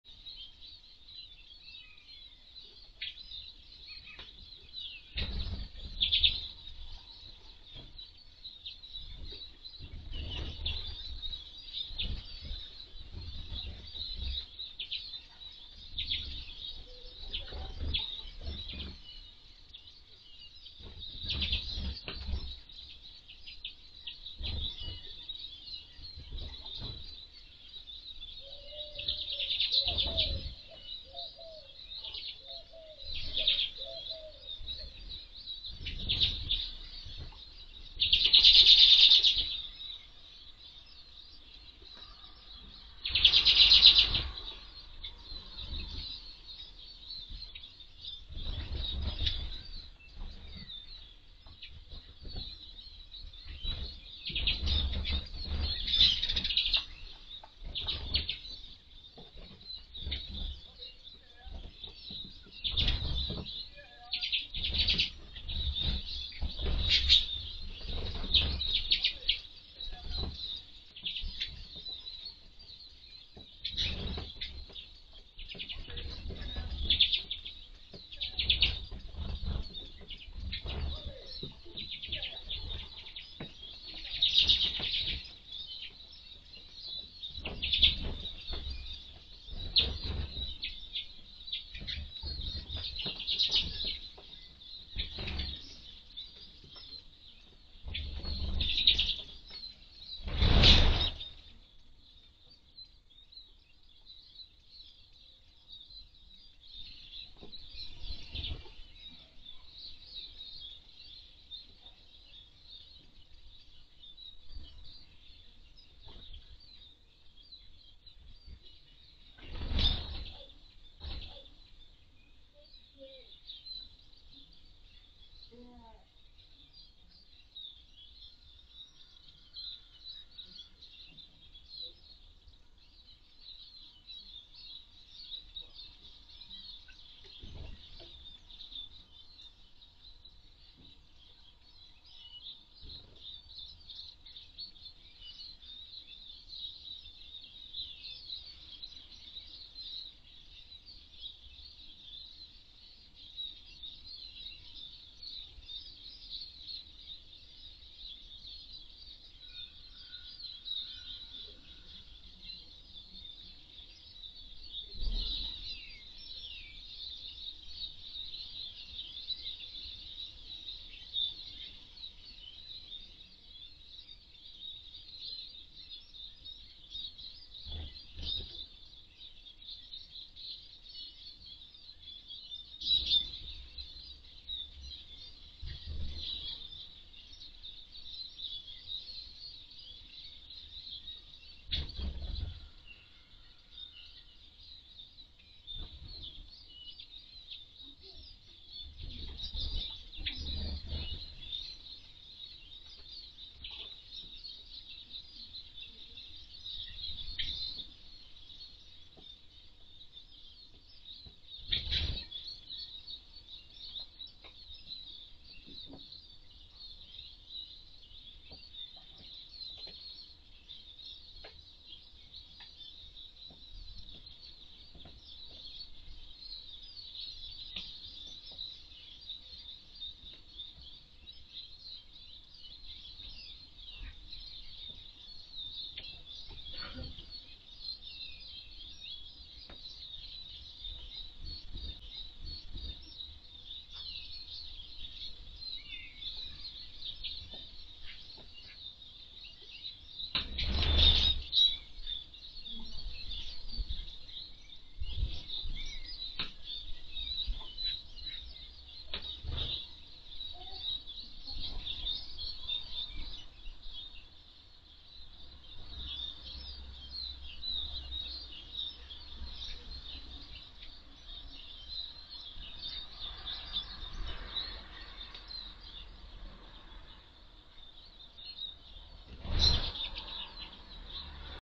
Erlenzeisige und Birkenzeisige an der Balkonfutterstelle.
Hier seht ihr einen Einblick in unsere offene Balkon-Vogelvoliere. Belebt mit Erlen,- und Birkenzeisigen. 😁😁😁